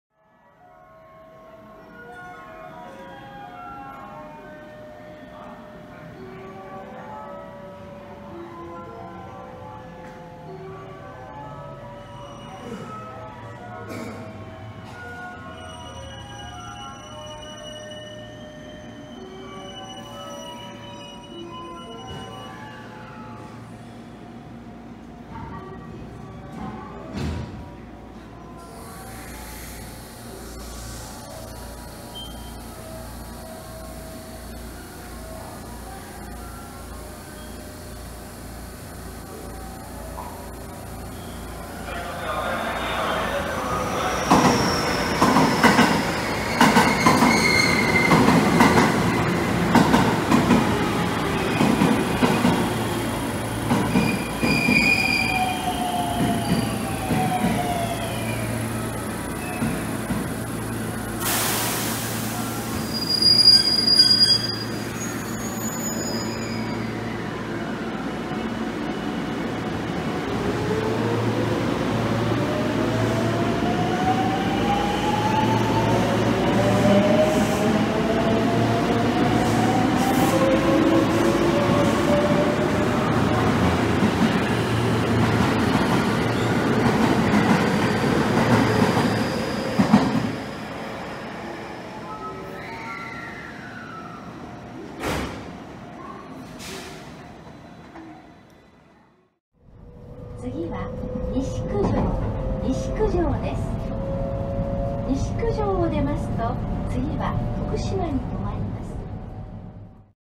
夜の阪和線堺市駅 関空/紀州路快速京橋行き到着と快速和歌山行き発車！
夜の阪和線堺市駅1番のりばに関空/紀州路快速京橋行が到着し、2番のりばを発車する快速和歌山行をする場面を収録。